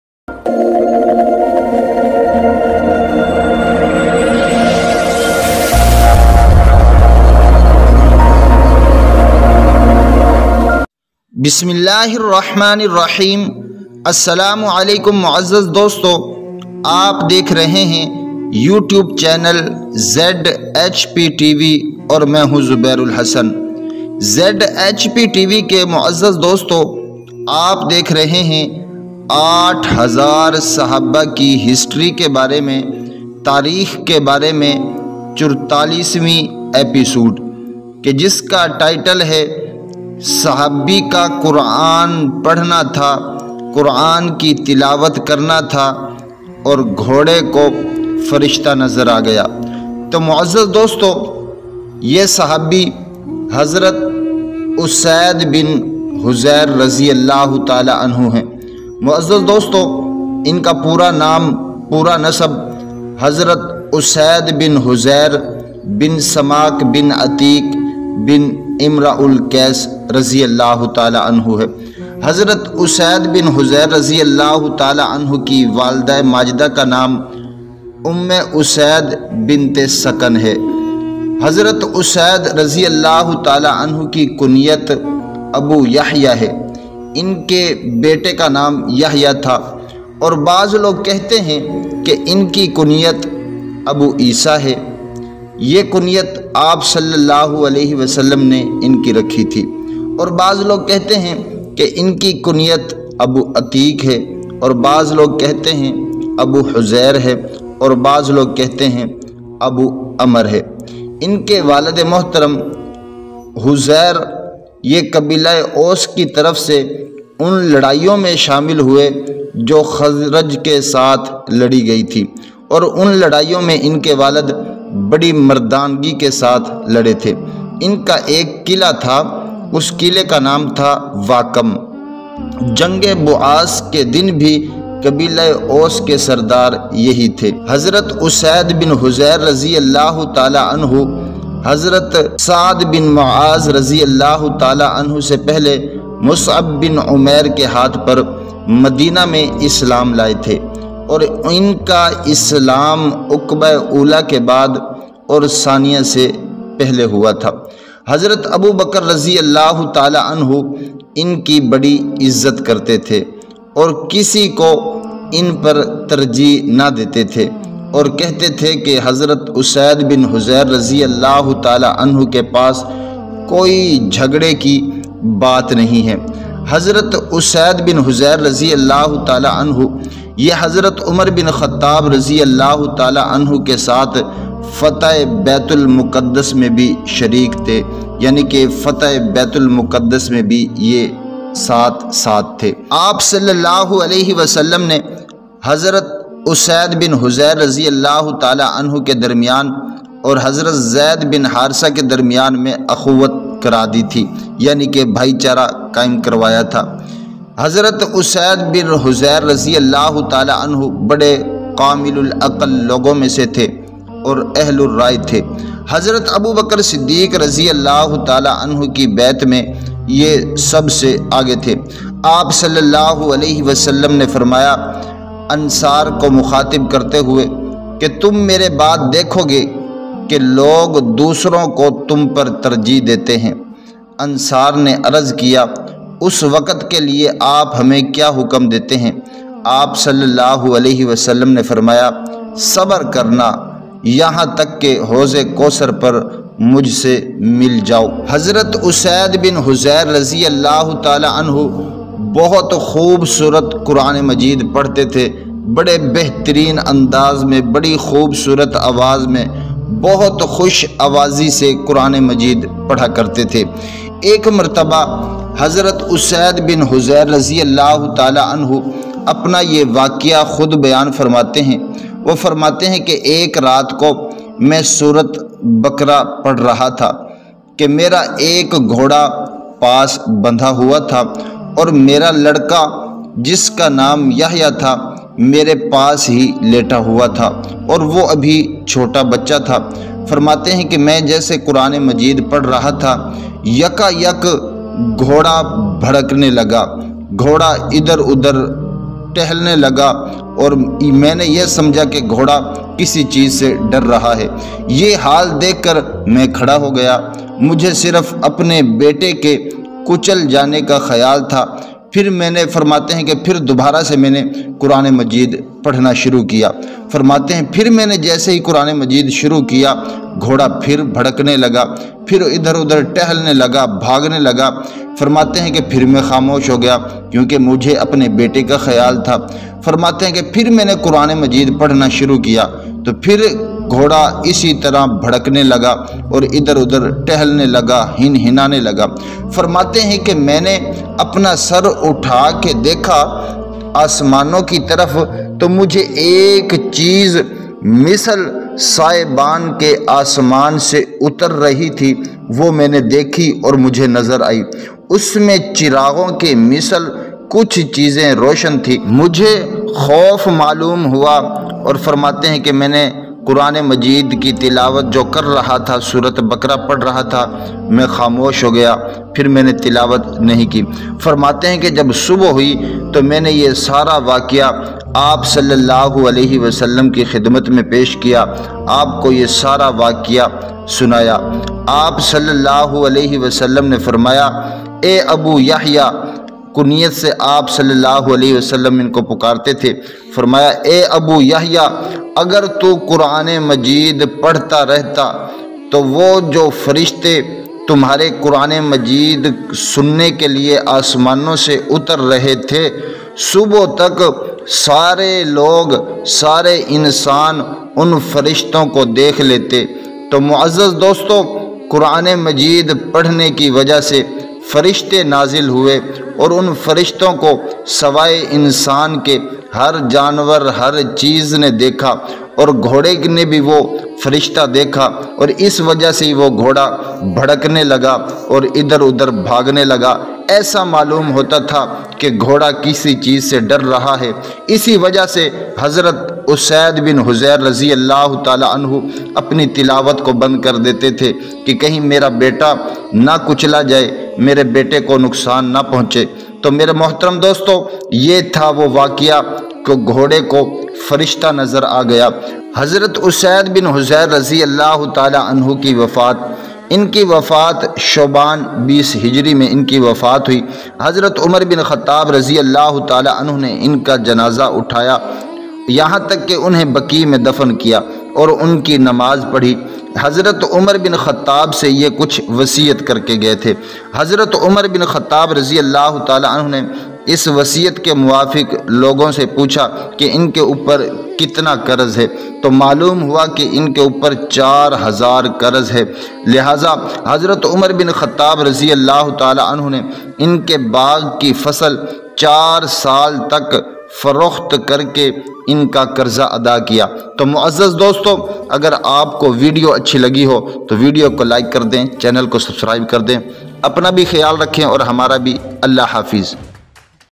Sahabi ka Quran Ki Tilawat karna tha ke Ghode ko Farishta Nazar A Gaya | 44 Episode | URDU HINDI
00: intro
04:25 Hazrat Usaid bib Huzair
08:36 wafat